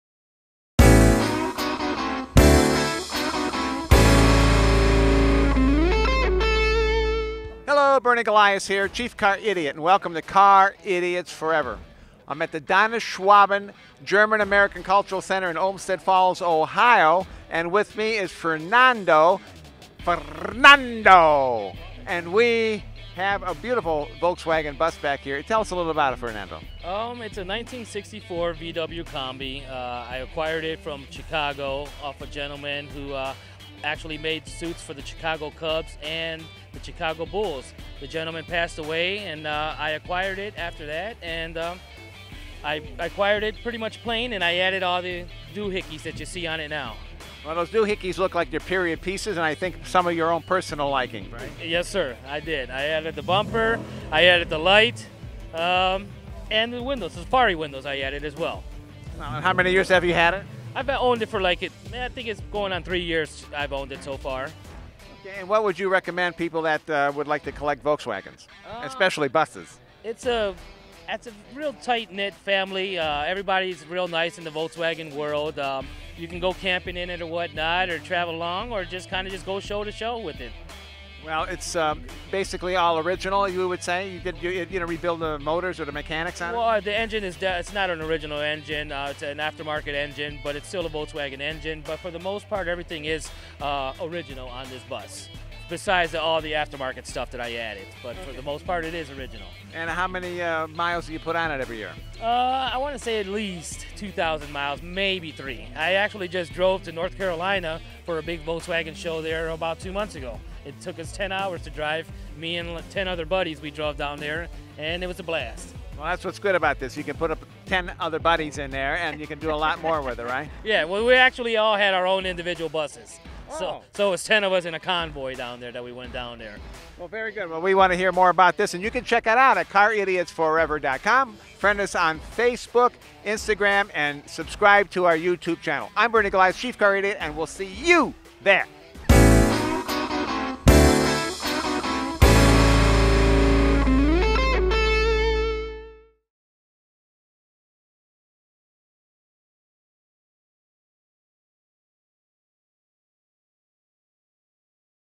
Fernando Torres and his 1964 VW Bus at the Euro Car Cruise-In
We're taken a road trip in STYLE 🛣

Fernando showcases his 1964 Volkswagen Kombi at the 2019 Euro Club Car show! Check out more awesome content from car shows near you and more at caridiotsforever.com!
video by West 10G Productions